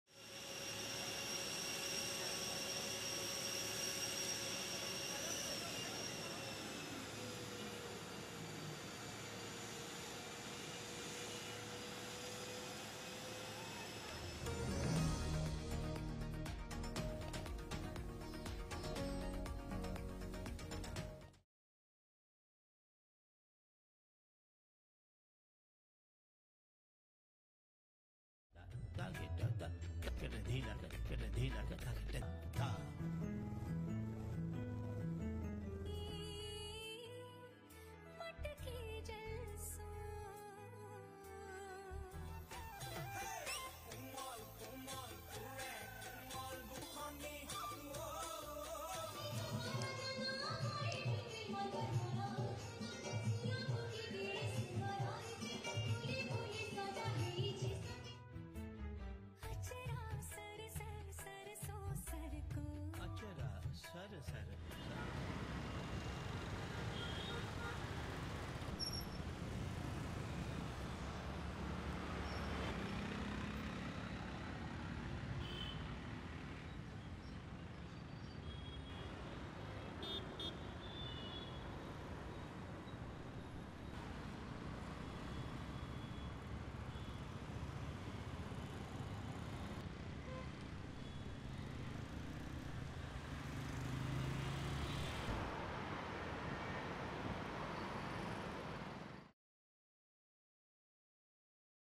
World leaders arrive in New Delhi for G20 summit
It is the first time India has hosted the forum